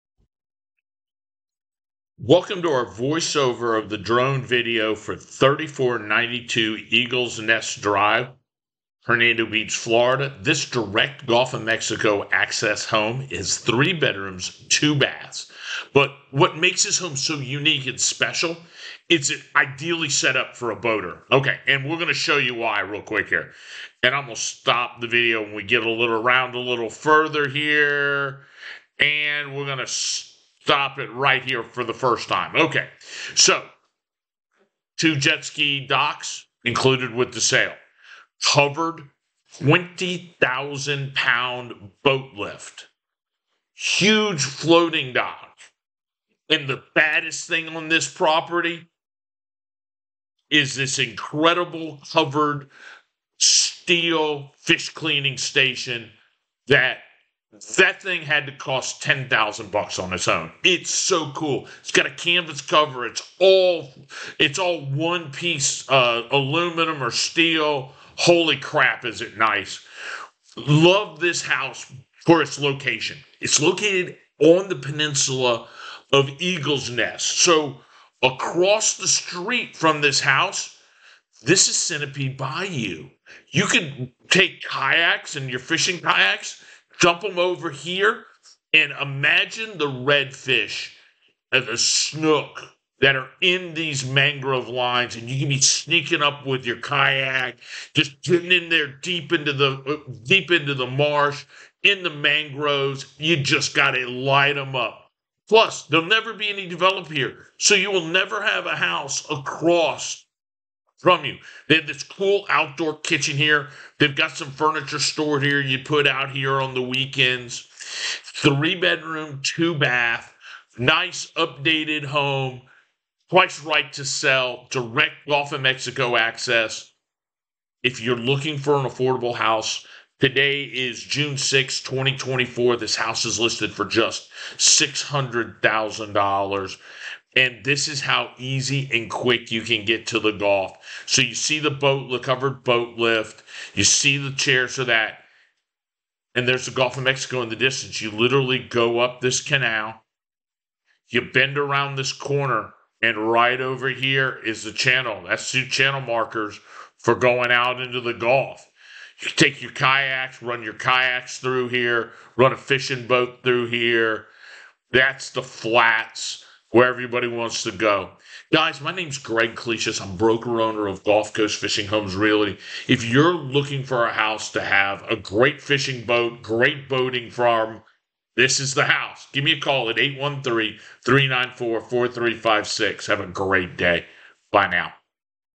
Voiceover of Drone Video for 3492 Eagle Nest Dr Hernando Beach FL 34607
For Sale: 3492 EAGLE NEST DR HERNANDO BEACH FL 34607 for $599,900

Are you dreaming of the perfect boating paradise? Look no further than this stunning Gulf Coast fishing home in Hernando Beach! Boasting 125 feet of waterfront on a saltwater canal with direct access to the Gulf of Mexico, this property is a boater's dream come true.

Features:

3 bedrooms, 2 bathrooms
Open floor plan
Updated and move-in ready
Covered 20,000-pound lift
Covered fish cleaning station
Fixed dock
Floating dock
2 jet ski docks
Outdoor kitchen
Quartz countertops
Updated bathrooms
Stainless steel appliances
All ceramic tile flooring for easy maintenance
This home offers everything you need for the ultimate boating lifestyle, including ample space for your watercraft and a covered fish cleaning station for your daily catch. Inside, the open floor plan is perfect for entertaining, while the updated features ensure modern comfort and convenience.

Direct access to the Gulf of Mexico means you can explore the open waters without any fixed bridges standing in your way. And with no HOA restrictions, you have the freedom to customize your waterfront oasis to your heart's content.

Welcome to Hernando Beach, a hidden gem along the Gulf of Mexico where recreational fishing and boating thrive. Nestled amidst a backdrop of tranquil coastal living, this community offers a unique blend of serenity and vibrancy.

While Hernando Beach may lack a traditional sandy shore, it more than compensates with its cool coastal vibe, waterfront tiki bars, marinas, and fine dining restaurants. The active commercial seafood industry ensures a steady supply of fresh catches, while world-class fishing experiences await just minutes from your doorstep.

When you're not out on the water, you can explore the nearby Weeki Wachee spring and river, perfect for swimming, paddleboarding, and kayaking. And with easy access to major highways and urban amenities, including shopping centers, restaurants, and hospitals, you'll never be far from civilization.

Experience the extraordinary Gulf Coast lifestyle in Hernando Beach, where the rhythms of the sea and the charms of coastal living converge in perfect harmony. Welcome home to paradise.